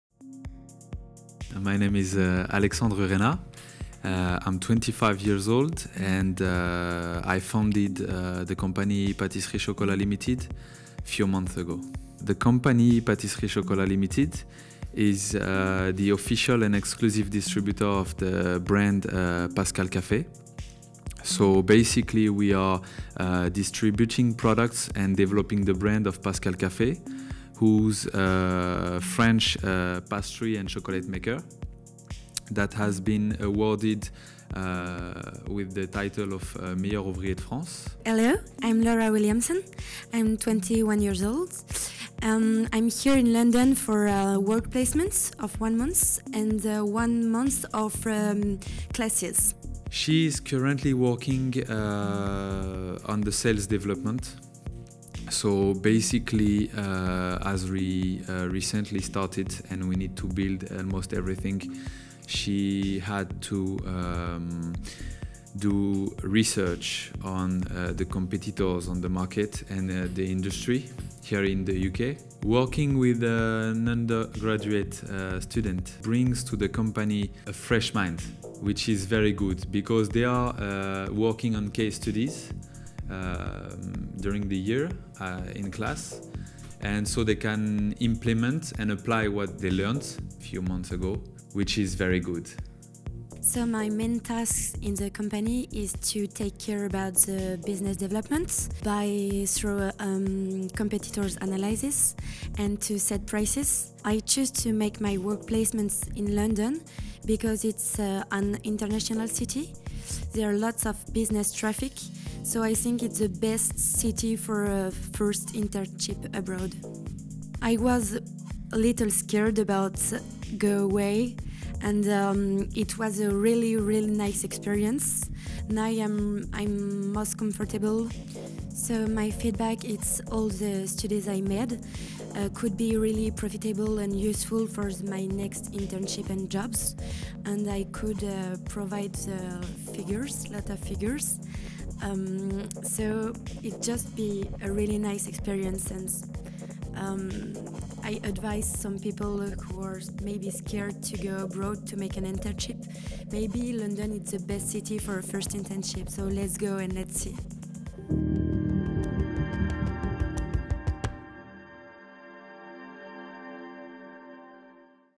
First steps into the professional world with Pascal Caffet UK | INSEEC London
INSEEC London speaks with Alexandre Urena, the official & exclusive distributor of Pascal Caffet in the UK about the company and the opportunities they have available for future employment as they expand their operations.

As part of their course, our Business Session students are in the midst of a 4 week work placement within companies in London in areas ranging from finance to human resources and marketing.